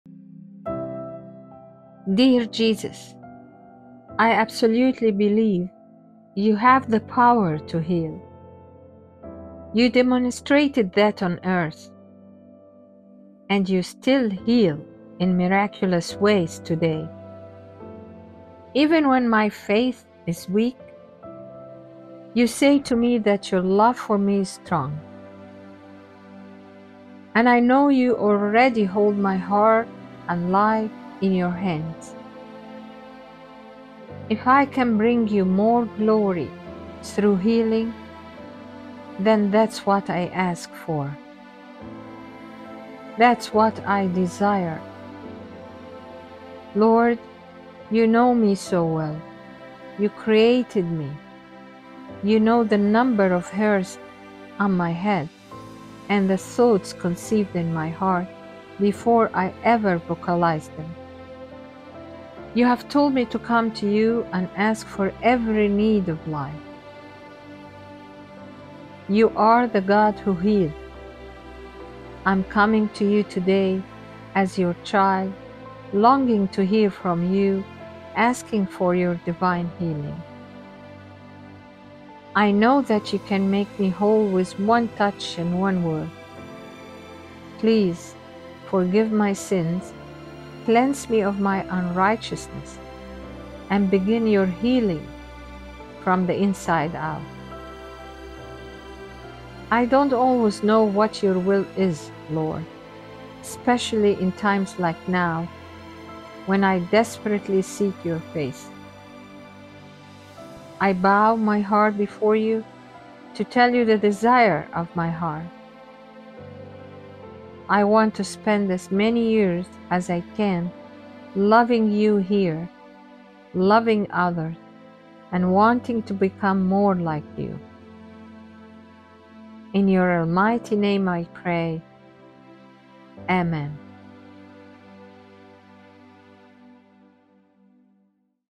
The Bread of Life | A Prayer For Personal Healing
Today's Prayer
🔴A Prayer For Personal Healing 🔴

🔴 The Bread of Life
Daily Devotionals- Prayers - Spiritual Growth - Words of Wisdom

🟡 If you want to receive The Bread of Life daily

Dear Jesus,

I absolutely believe You have the power to heal.
You demonstrated that on earth, and You still heal in miraculous ways today.
Even when my faith is weak, You say to me that Your love for me is strong.
And I know You already hold my heart and life in Your hands.
If I can bring You more glory through healing, then that's what I ask for.
That's what I desire.

Lord, you know me so well.
You created me.
You know the number of hairs on my head and the thoughts
conceived in my heart before I ever vocalize them.
You've told me to come to You and ask for every need of life.
You are the God who heals.
I'm coming to You today as Your child, longing to hear from You
and asking for Your divine healing.
I know that You can make me whole with one touch and one word.

Please forgive my sins, cleanse me of my unrighteousness,
and begin Your healing from the inside out.
I don't always know what Your will is, Lord, especially in times like now,
when I desperately seek Your face.
I bow my heart before You to tell You the desire of my heart.
I want to spend as many years as I can loving You here, loving others,
and wanting to become more like You.
In Your Almighty name I pray.
Amen.